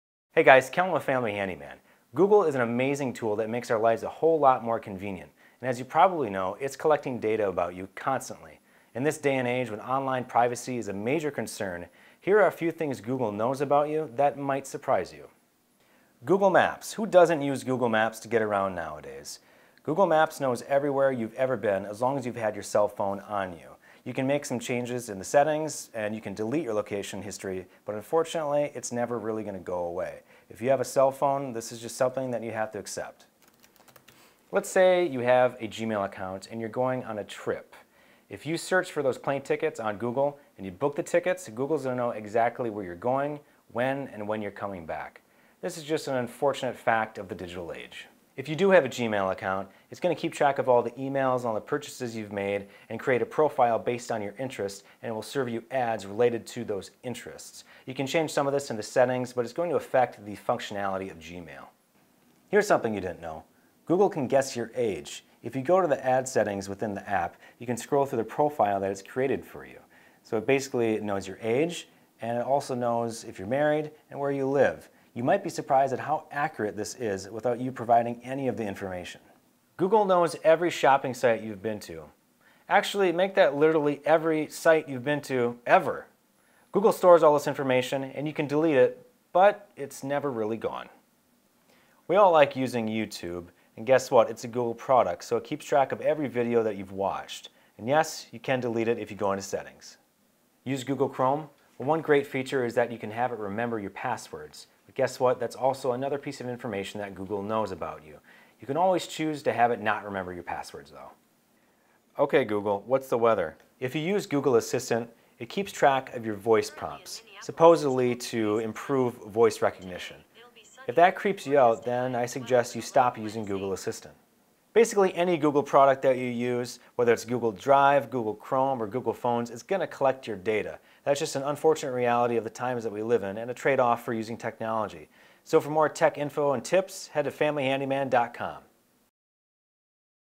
Things Google Knows About You
You're not gonna believe all the info Google has on you...but it's true. And here's what you can do about it.